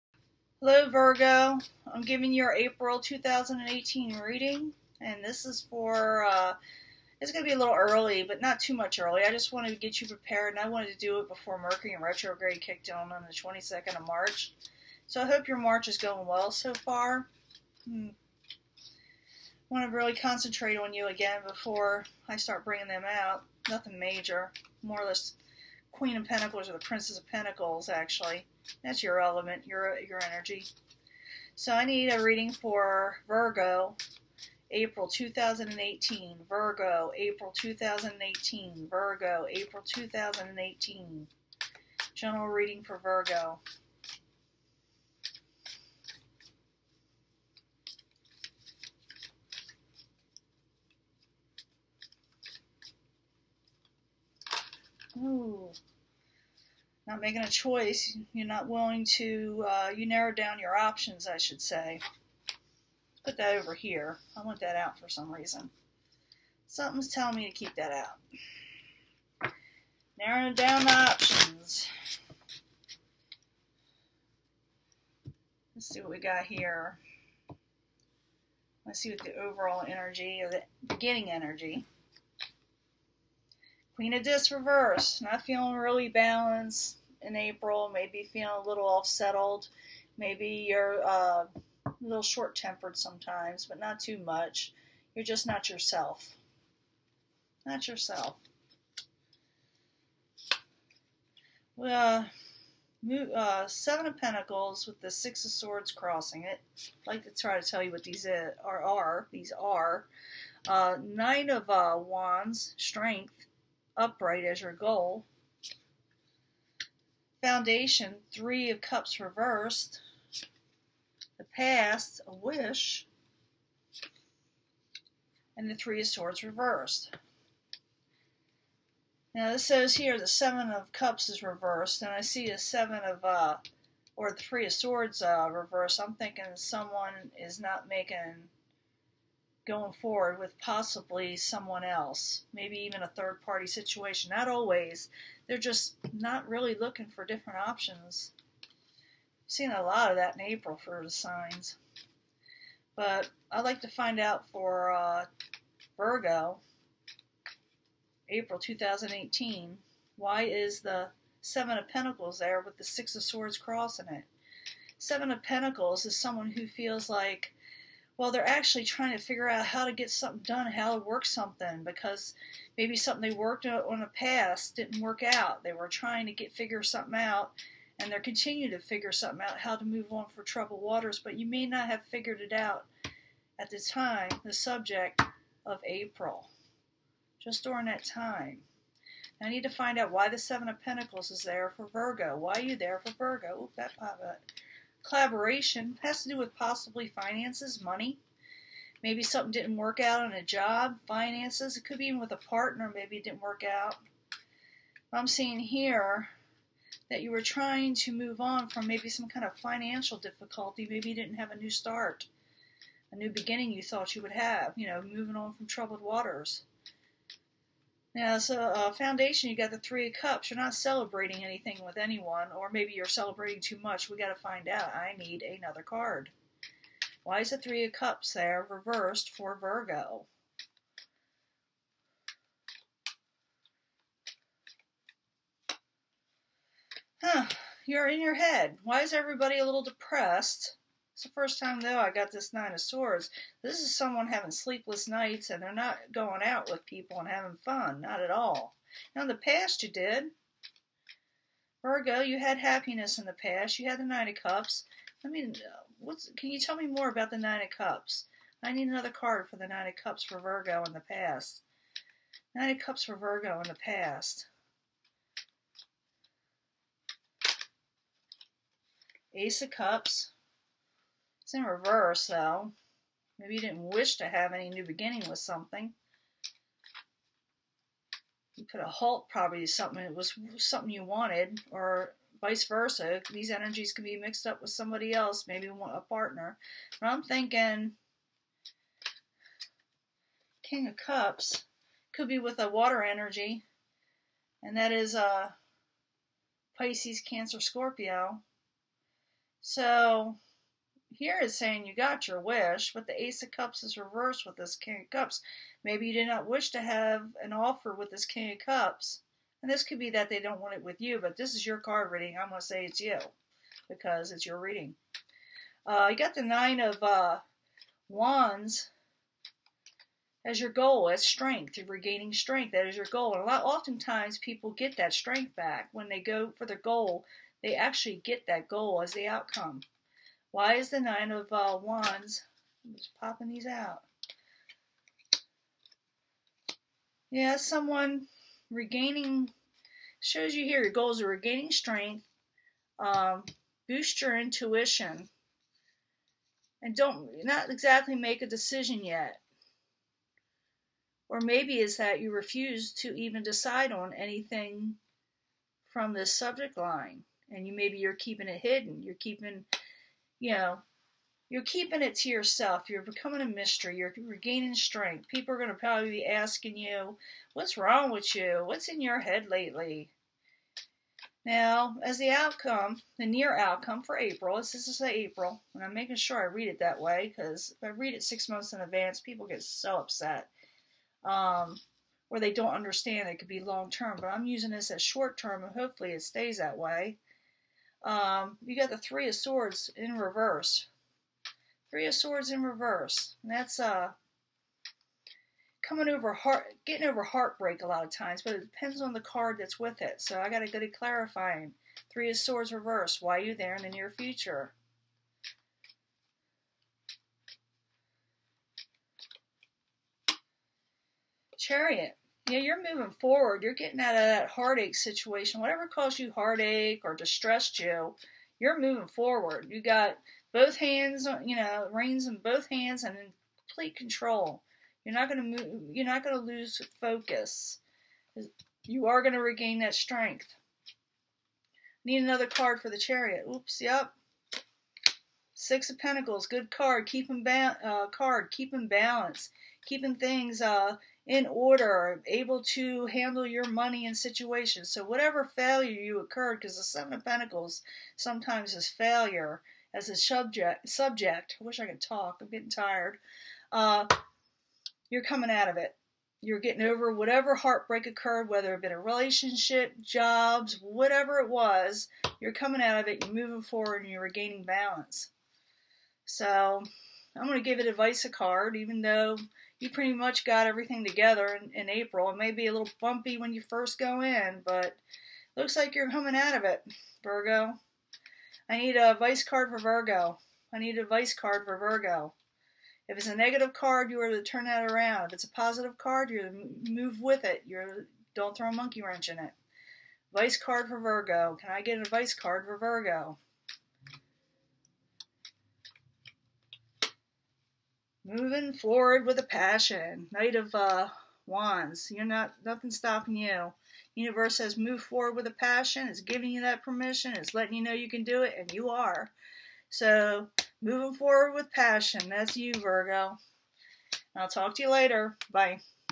Virgo, Great Strength, Balance and Passion! April 2018 Tarot Reading!
***I try to show the cards for those who are hearing impaired.